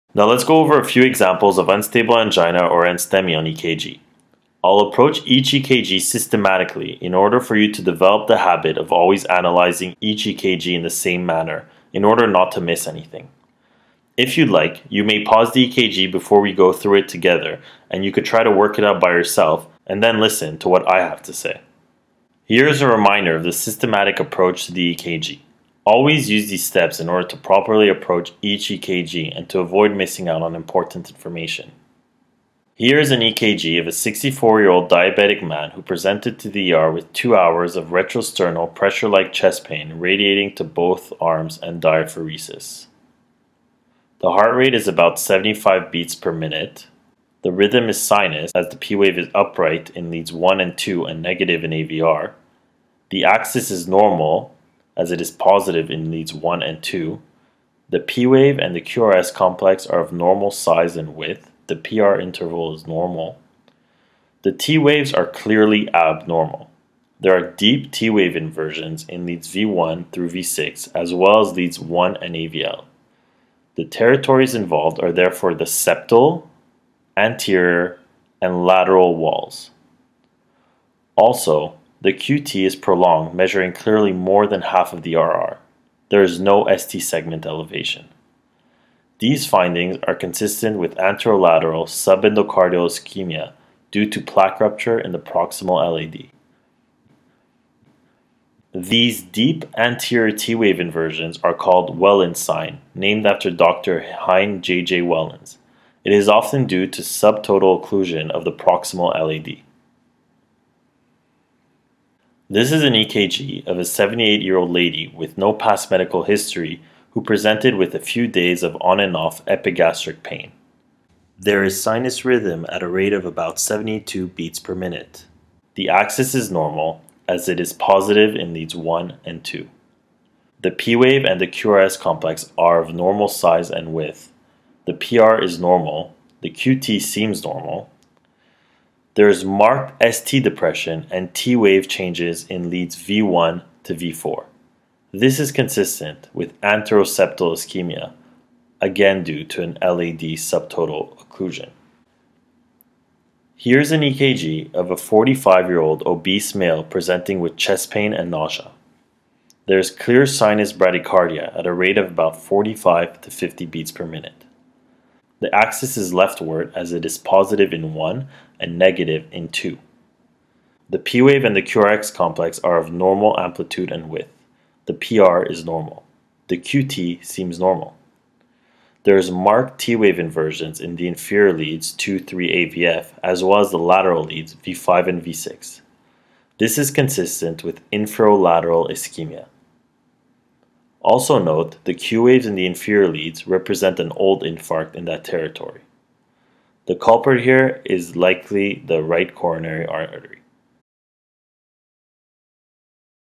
Ischemia 5/7 -Examples of UA and NSTEMI
In our fifth video, we provide examples of UA/NSTEMI on ECG. We review a stepwise approach to interpreting the ECG in patients presenting with UA/NSTEMI.